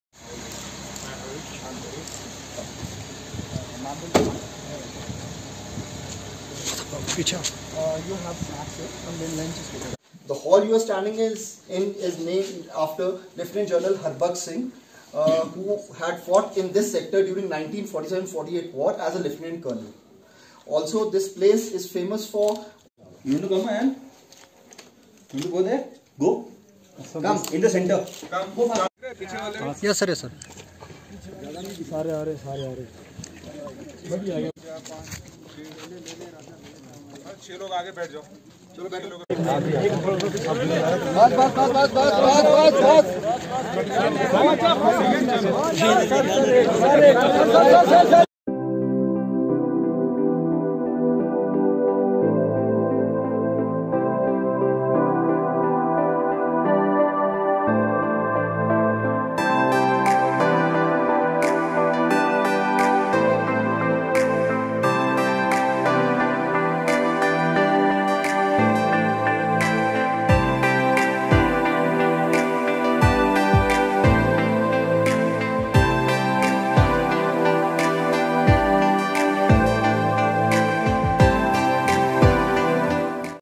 Former Indian Cricketer, Yusuf Pathan visit Tanghdar Valley of North Kashmir's Kupwara
Former Indian Cricketer, Yusuf Pathan visit Tanghdar Valley of North Kashmir's Kupwara district, and interact with Indian Army and locals.